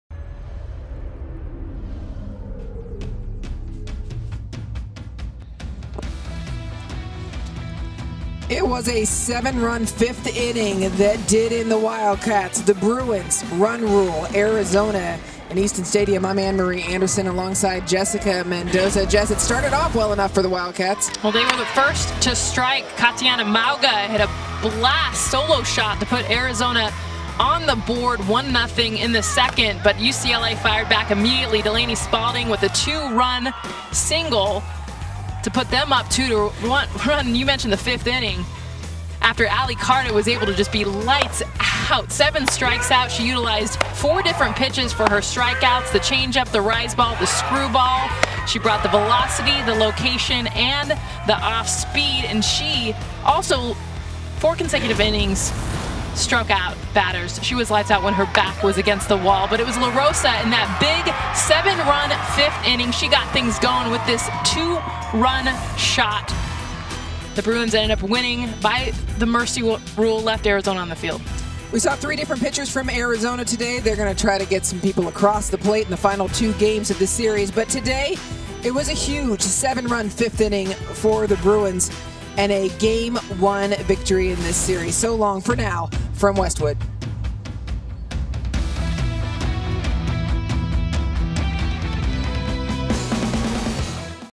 No. 7 UCLA Softball Wins Big Over No. 16 Arizona
(Originally published on 05/02/2015) Pac-12 Networks' Anne Marie Anderson and Jessica Mendoza recap Friday night's 9-1, run-rule victory by the No. 7 UCLA softball team over No. 16 Arizona on Friday in Westwood.